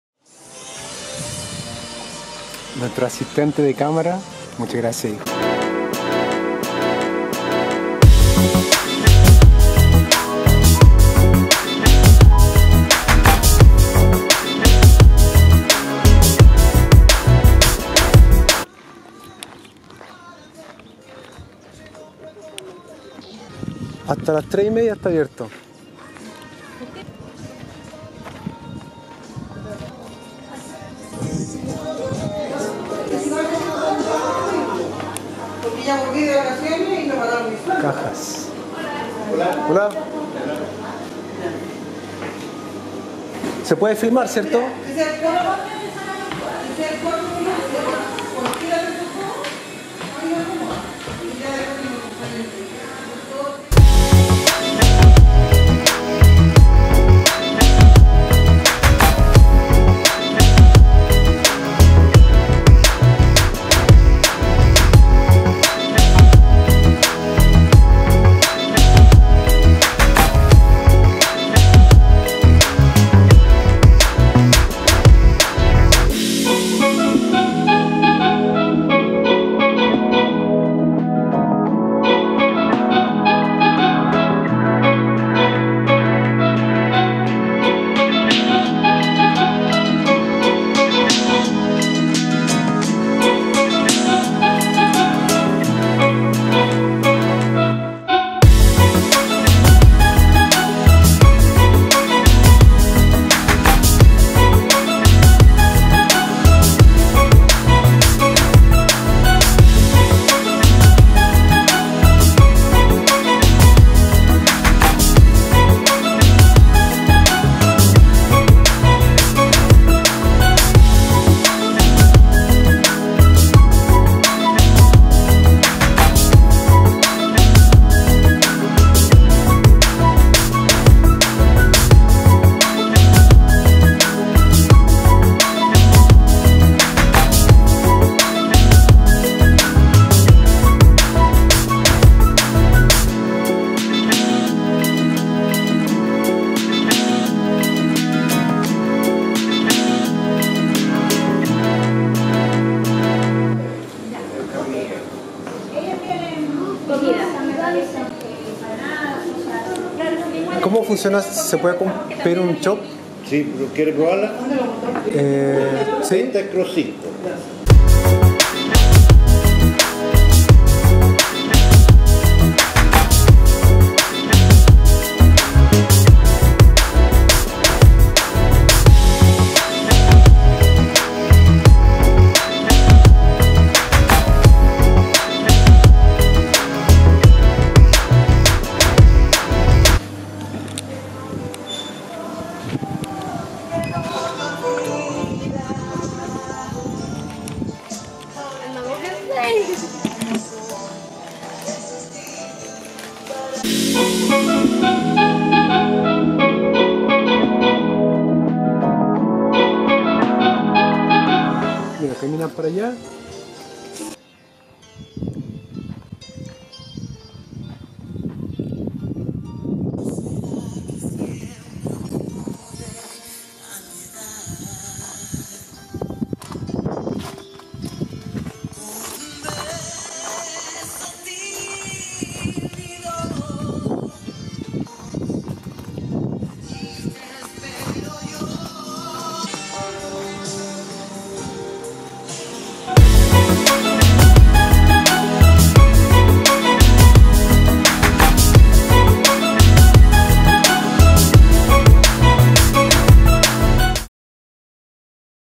Visitando Planta de Cervecería Kross, vlog_023
Estuvimos de paso en la planta de Cervecería Kross en Curacaví, su sala de ventas fue una agradable sorpresa!